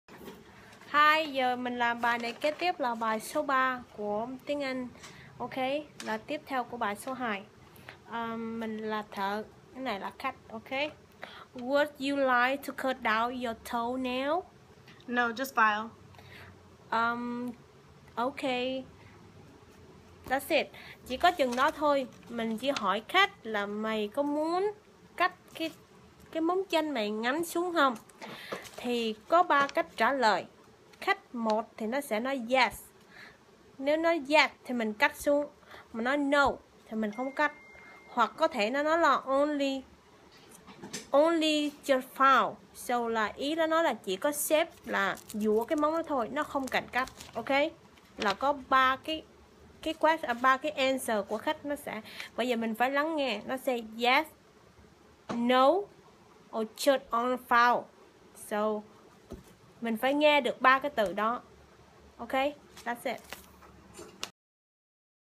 Hướng dẫn tiếng anh cơ bản cho nghề nails #3
Đây là video hướng dẫn tiếng anh cơ bản của nghề nails cho những người mới thứ 3 của Nisa. Rất mong nhận được mọi ý kiến phản hồi của mọi người để Nisa có thể làm tốt hơn.

  Bài số 3:
Thợ: Would you like to cut down your toe nails? (bạn có muốn cắt bớt móng của mình không?)
         guốt du lai tu cớt do thâu neo sờ
Khách: Yes/no/ only just file (vâng/không/chỉ dũa móng thôi)
             dét/ nâu/ on lì chợt phao
Thợ: okay
        âu khay